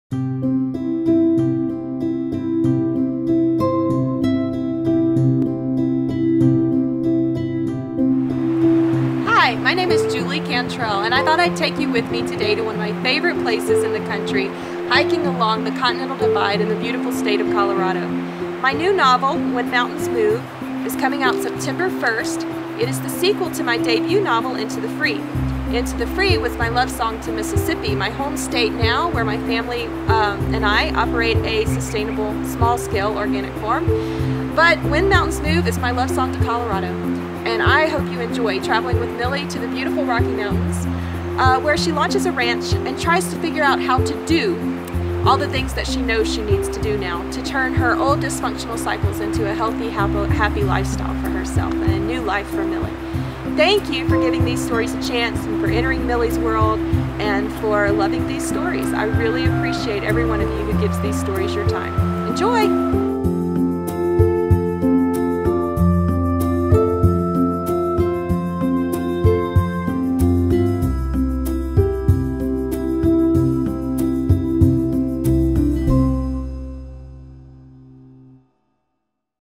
When Mountains Move by Julie Cantrell
"When Mountains Move"—Julie Cantrell's highly-anticipated sequel to "Into the Free"—is a novel of dark secrets, deep hope and second chances. For Millie, being truly free feels about as impossible as the mountains moving. But she's about to discover the one thing bigger than her past ... is her future.